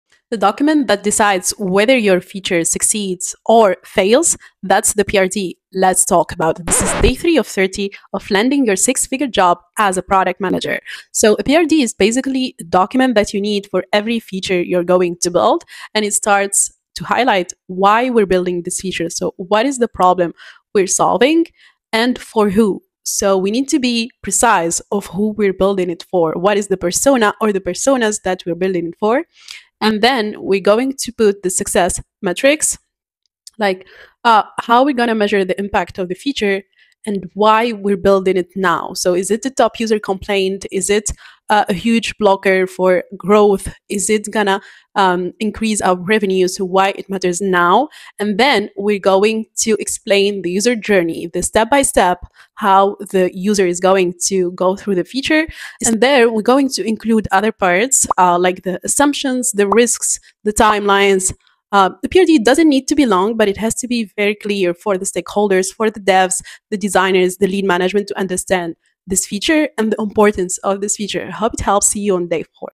Day 3/30 of landing your 6 figure job: the PRD (Product requirement document)
Day 3/30 — Let’s talk about the PRD (Product Requirements Document).
It’s not as scary as it sounds — it’s just the document that tells your team what you’re building and why.
Here’s the simple structure I use.